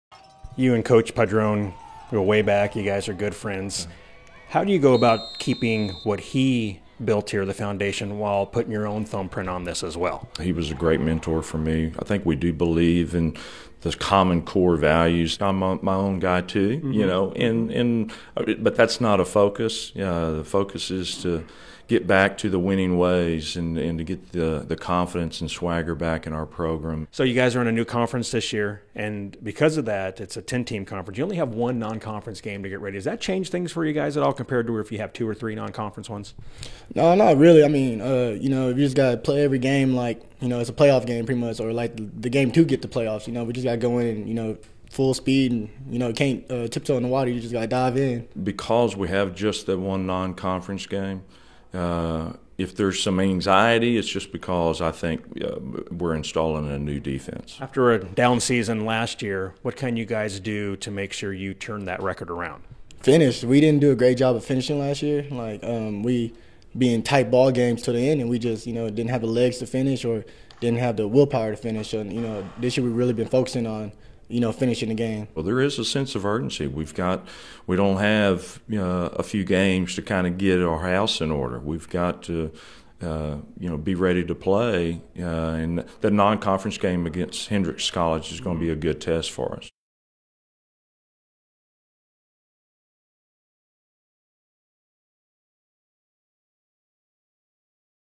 Season Preview: TLU brings new swagger, confidence into new conference
TLU is looking bring their new-found swagger into their new conference for the 2017 season.